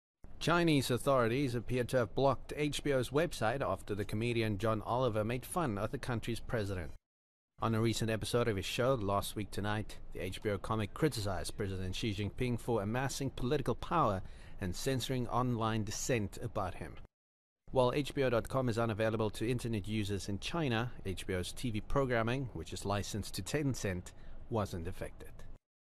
HBO Website Is Down in China After John Oliver Skewers Xi
Chinese authorities appear to have blocked U.S. film and television production company HBO’s website after comedy show “Last Week Tonight with John Oliver” aired a segment criticizing President Xi Jinping for censorship.